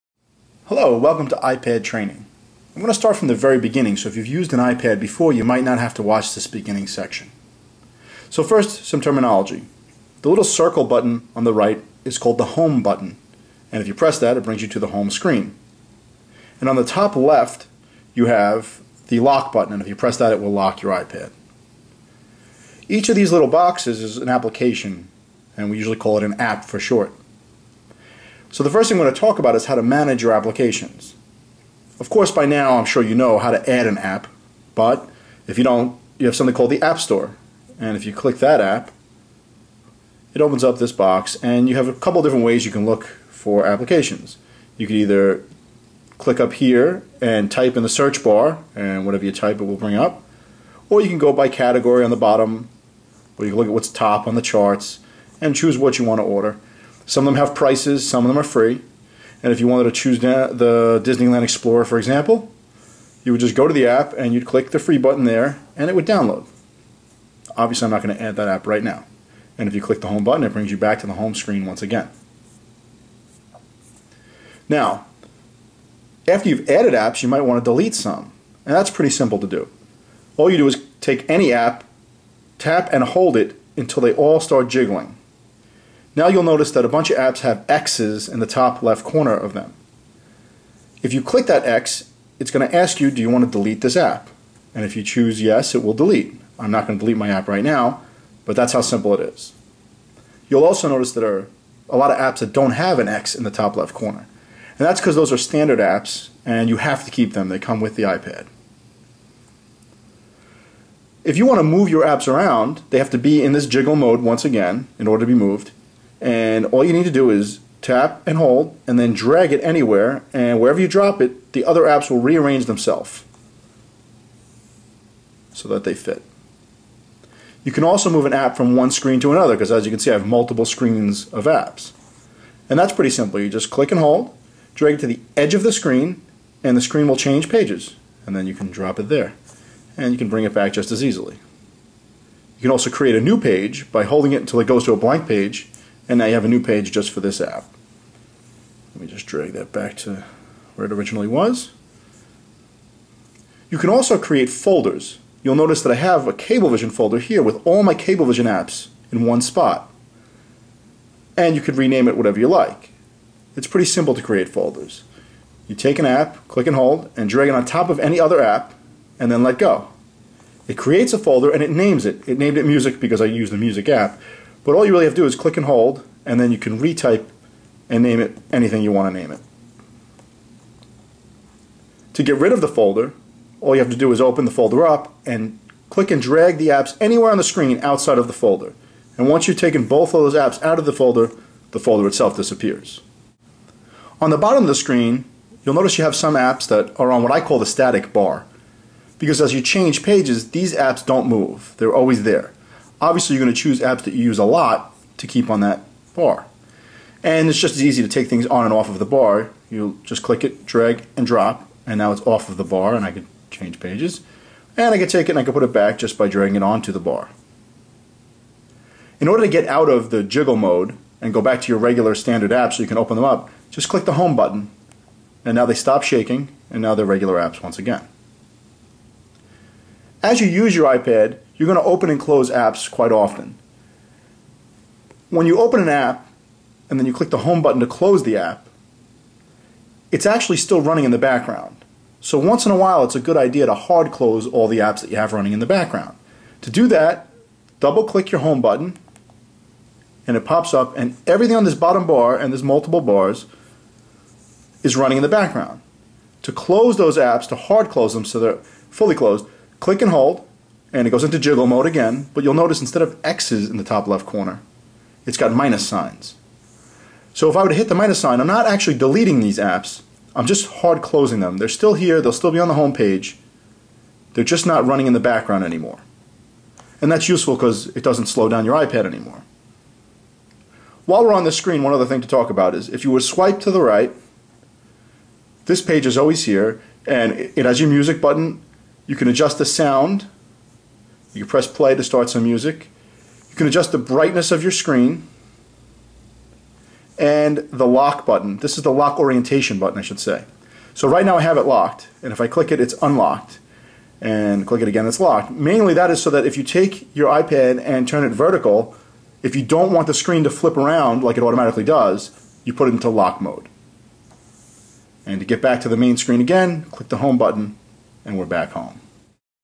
iPad_Basics.mov
This video is for a first time user of an iPad.  It discusses the very basic functionality of an iPad 2.